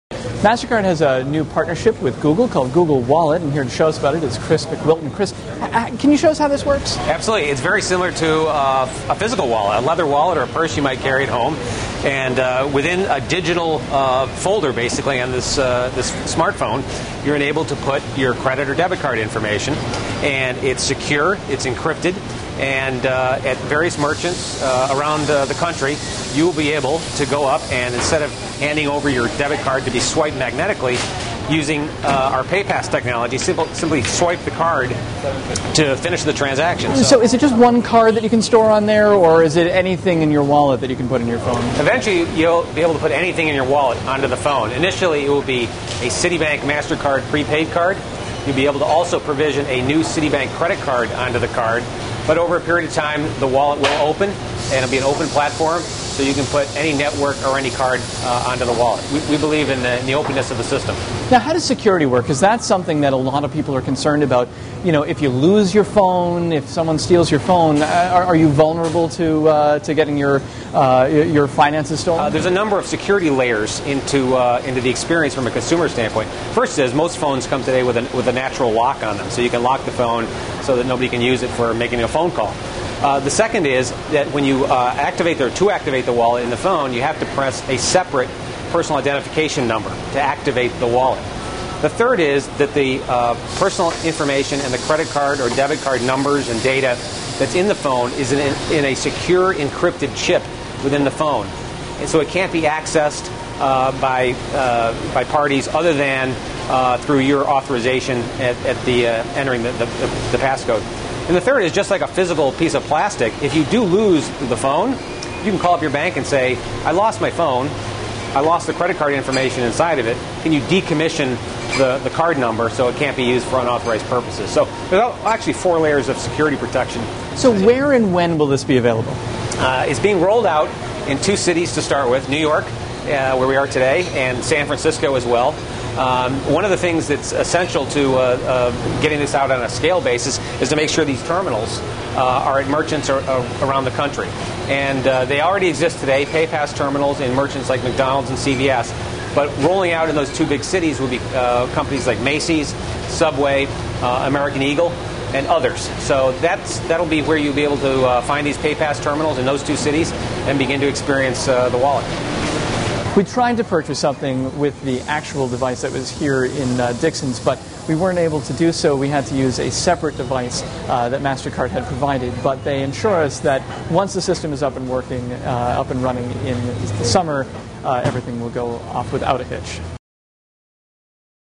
Google's mobile wallet is here
Google and MasterCard unveiled a mobile payment system Thursday that uses consumers' cell phones to store credit cards, coupons, and other data.